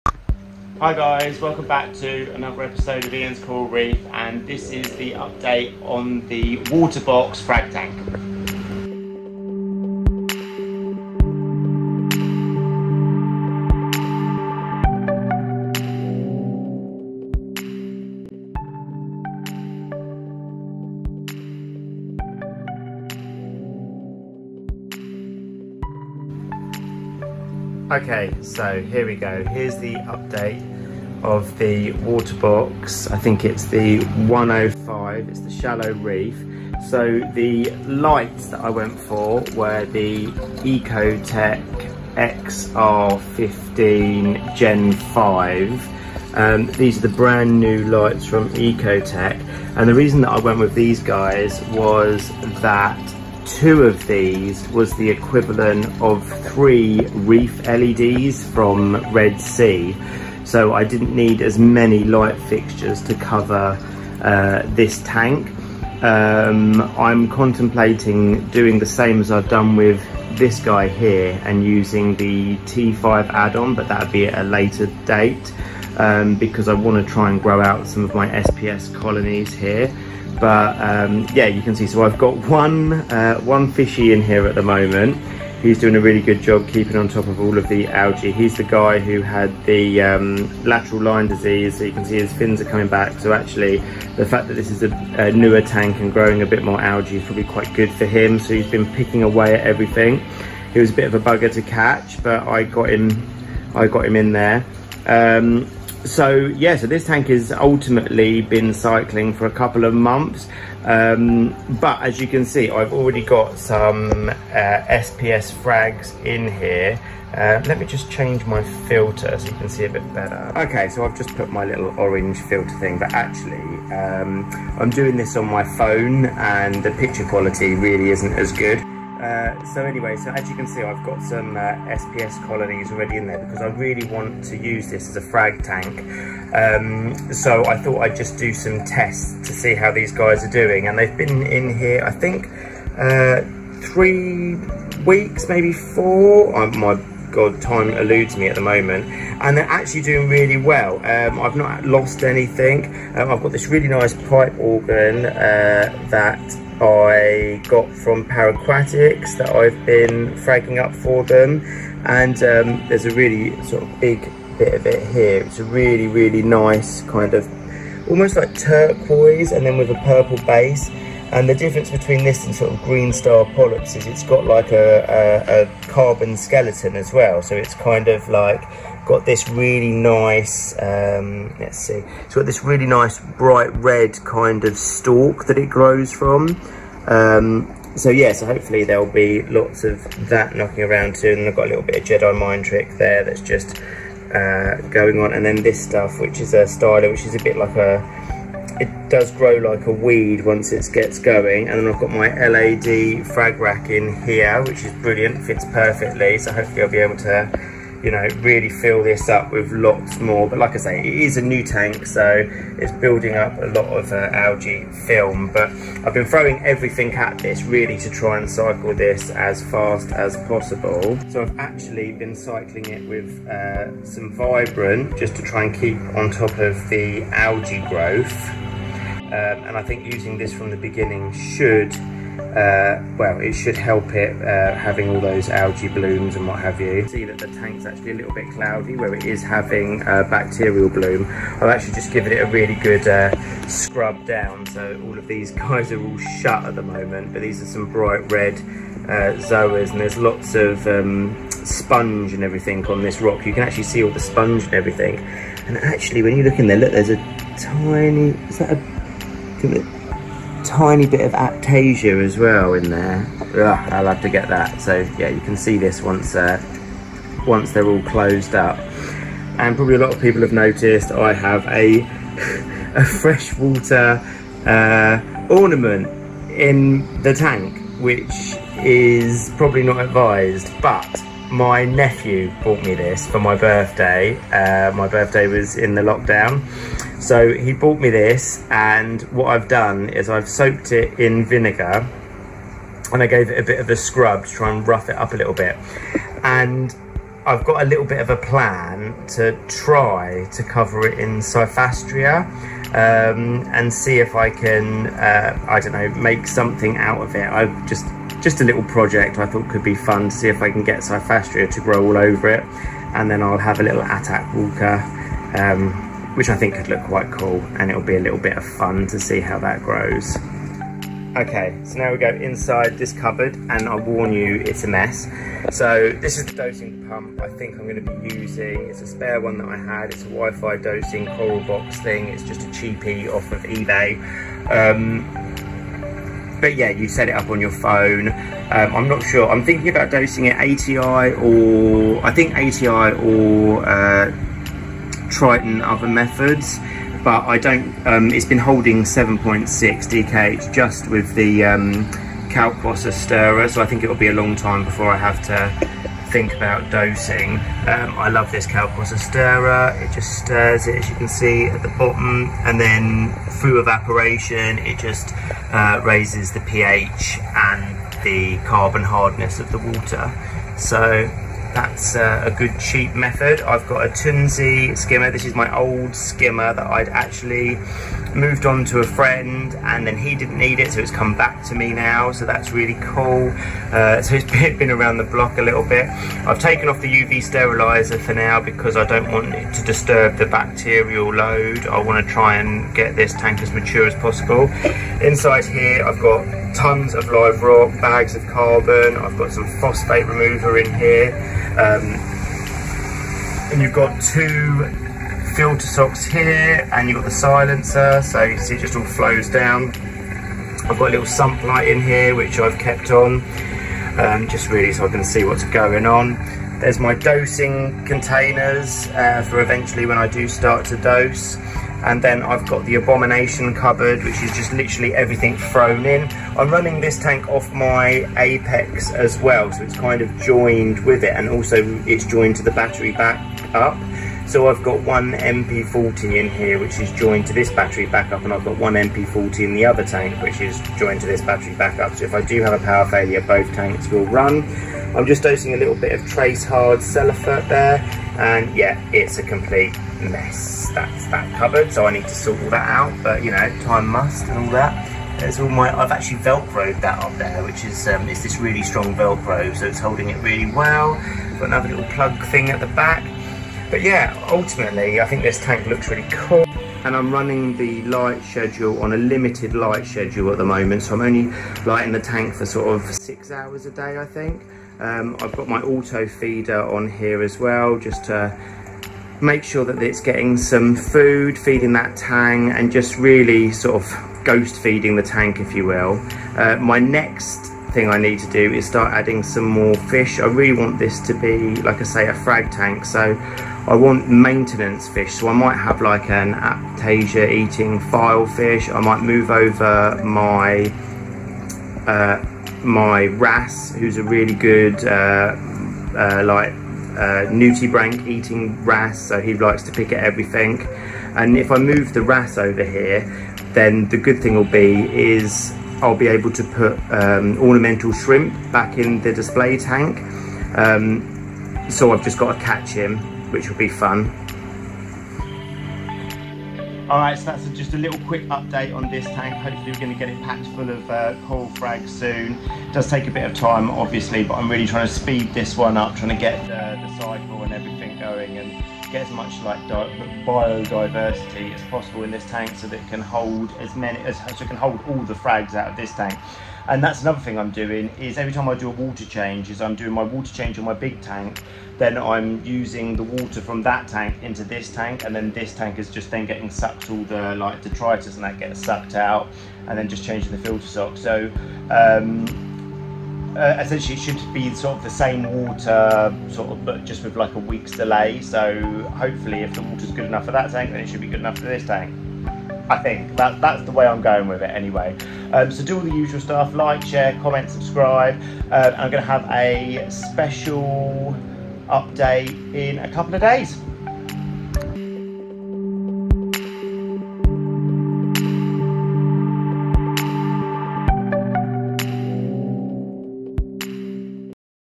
Waterbox 105 frag tank update
Lets have a look at my newest tank and see how well its doing !